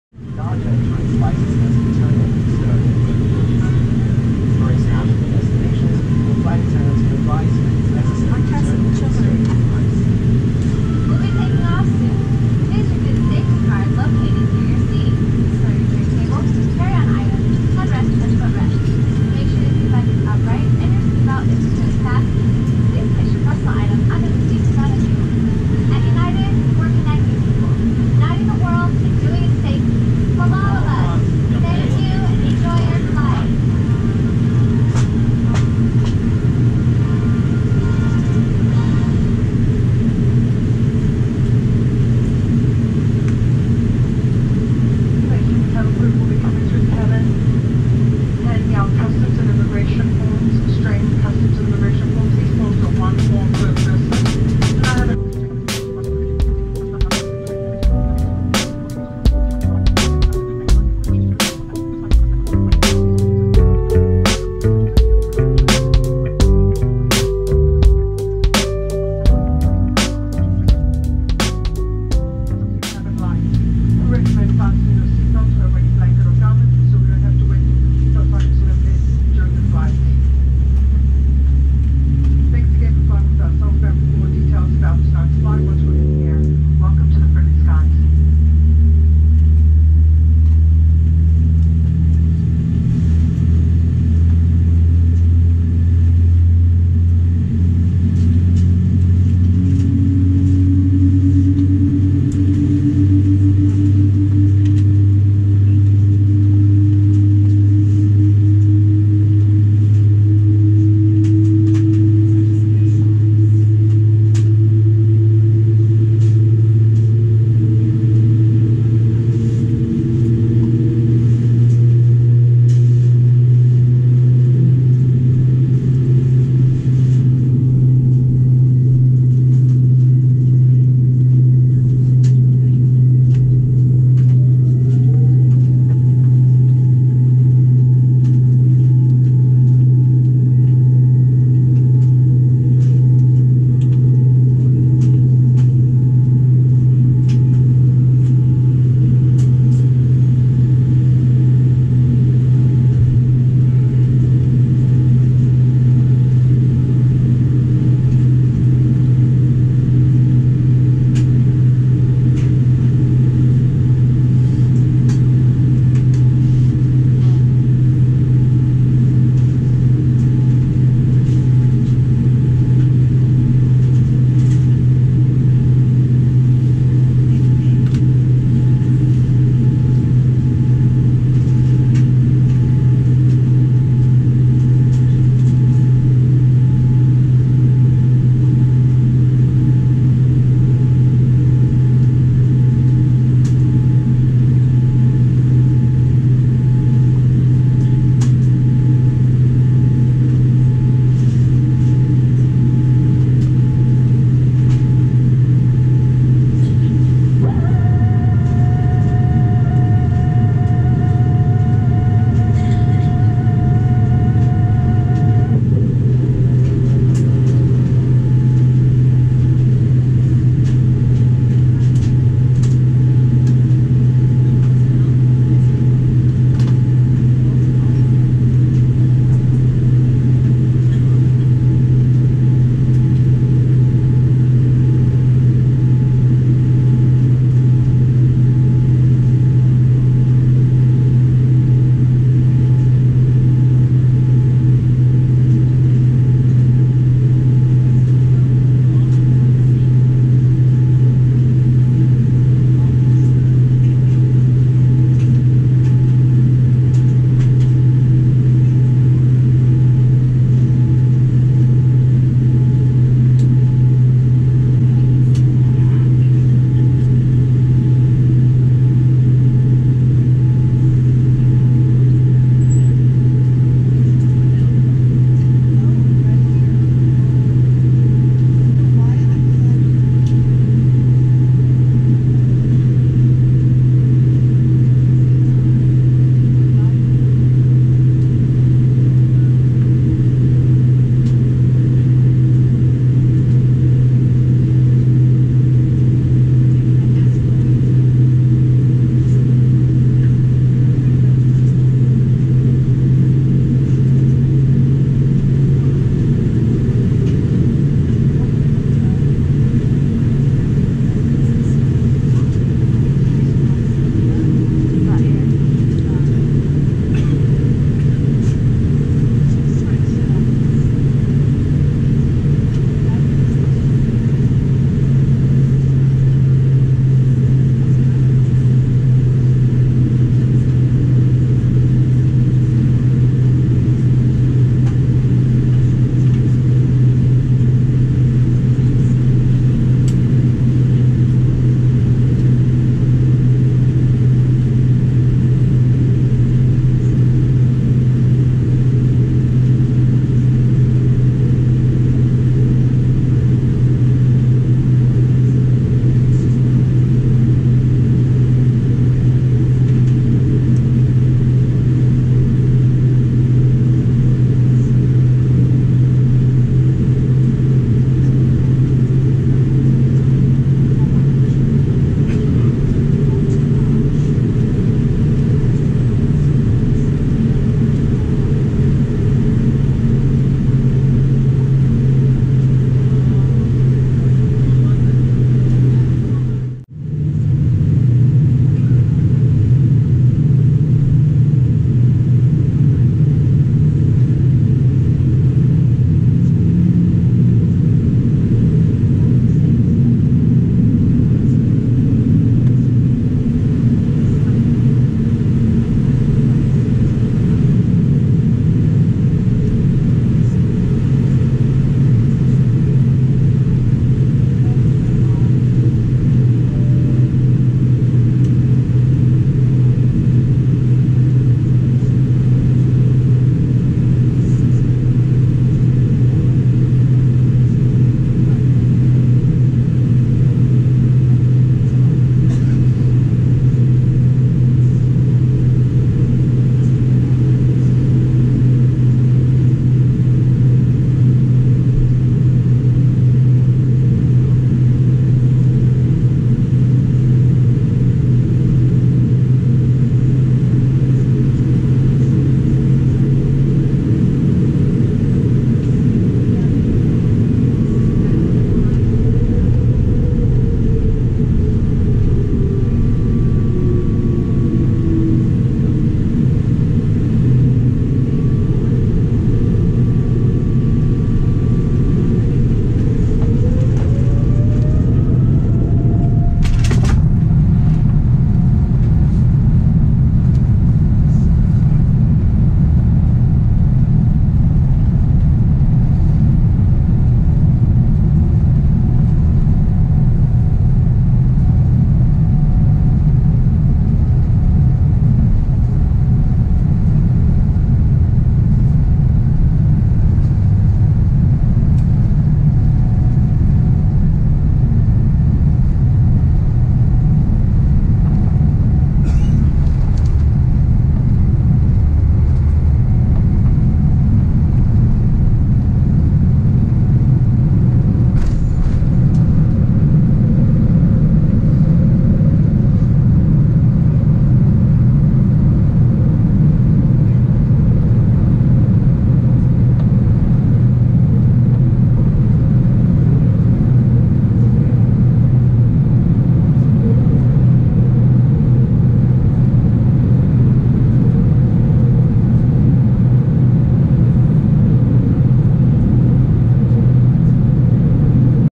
United Airlines Flight 863 (787-9) Takeoff
Hello everybody and welcome to this video.  Today, we will be departing out of San Francisco International Airport in a Boeing 787-9 operated by United bound for Sydney.  The view is from seat 7A.